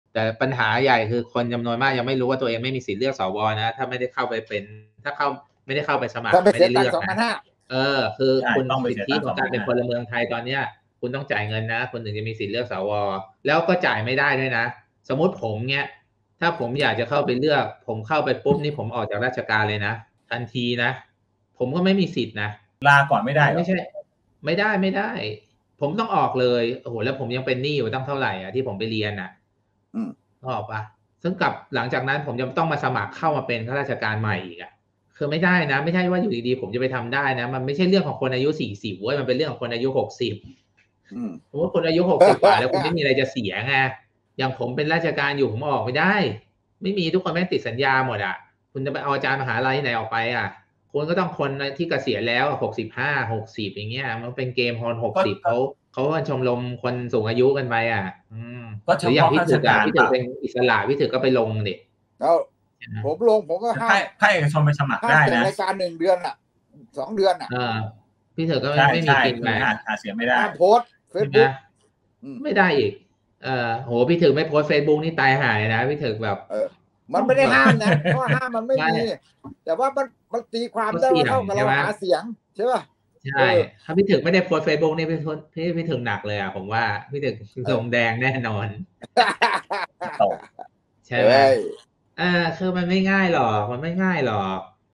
ถ้าไม่ได้สมัครเสียเงิน 2,500 ไม่มีสิทธิเลือก สว. | PrachaTalk EP.28
ถ้าไม่ได้สมัครเสียเงิน 2,500 ไม่มีสิทธิเลือก สว.

รับชม "PrachaTalk EP.28 : ประชามติ รธน. ไม่แตะหมวด 1, 2"